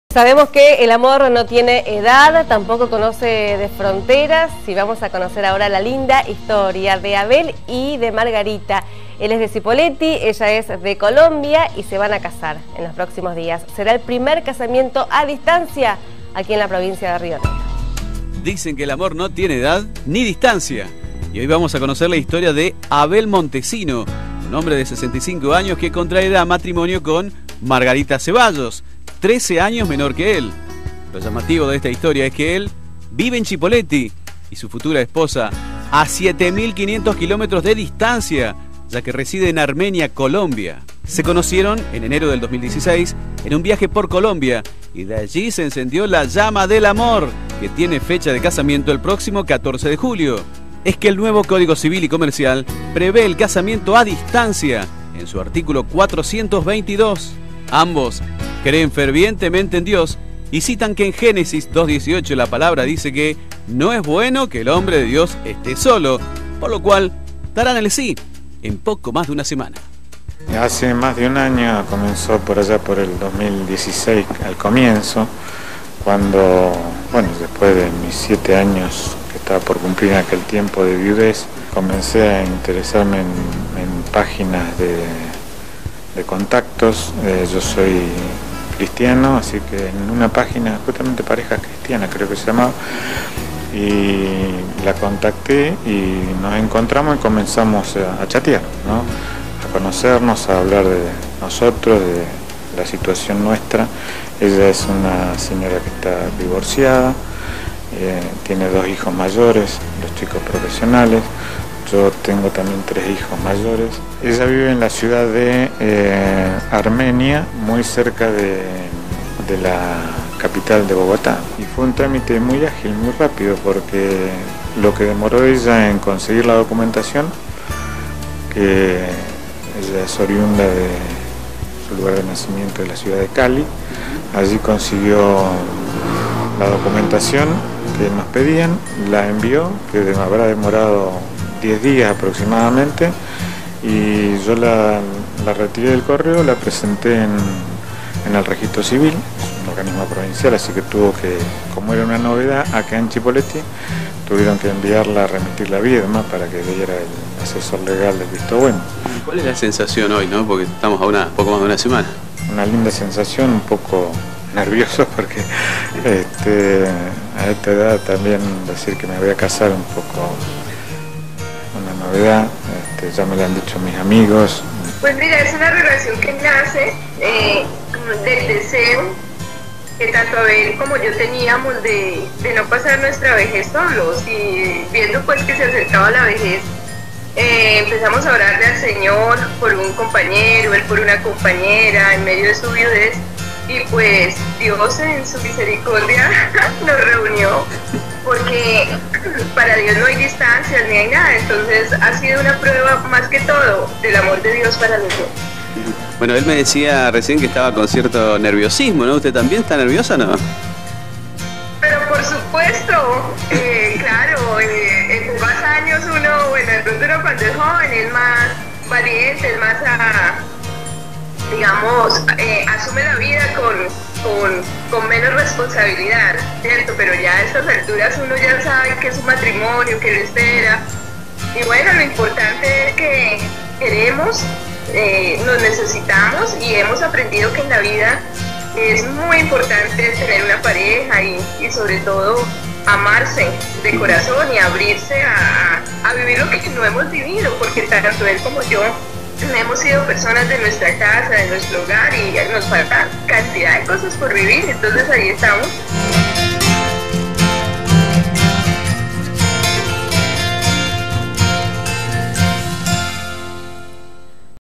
Cipolletti: casamiento a distancia
El artículo 422 del Código Civil, establece el casamiento a distancia. por lo que un hombre argentino se casará con una mujer colombiana.